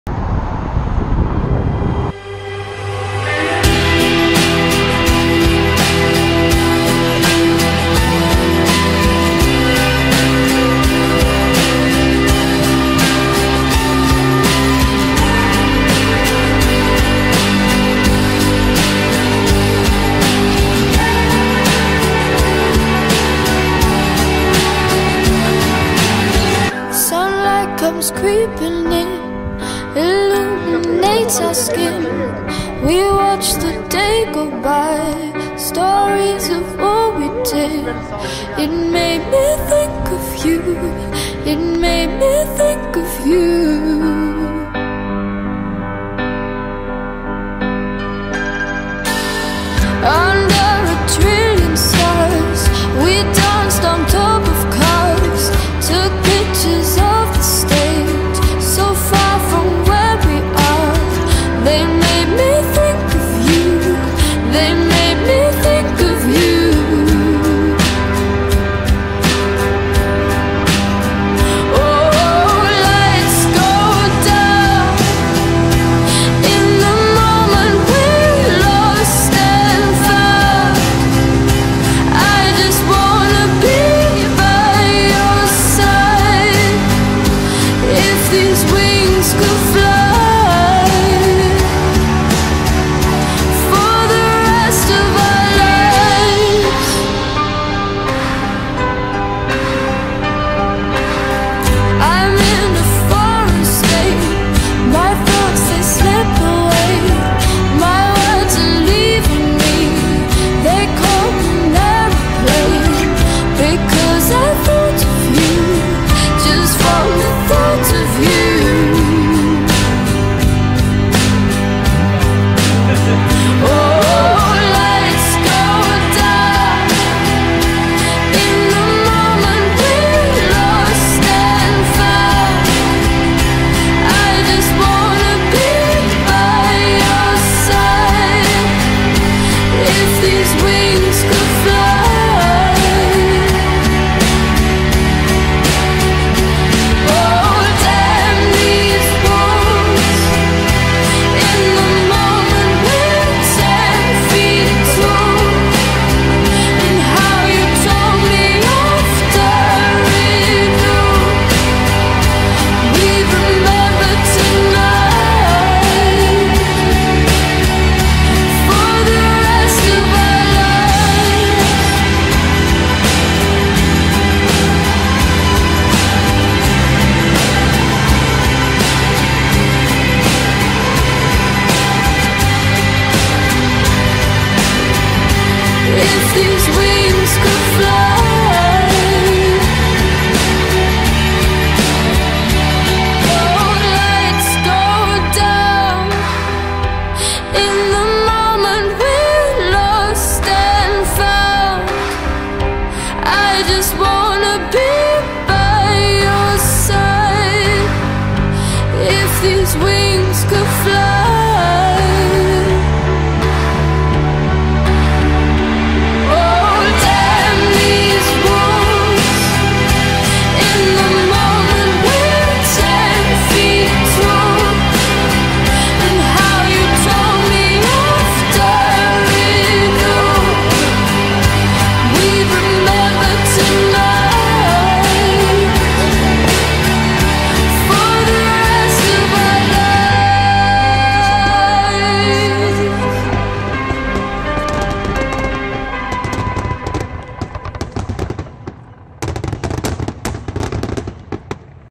Life as a Student Pilot at Hillsboro Aero Academy
My routine as a student pilot from Brazil in Hillsboro Aero Academy campus of Hillsboro (OR)